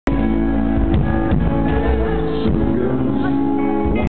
Jarvis Cocker singing to the crowd
Live @ Amsterdam Music Hall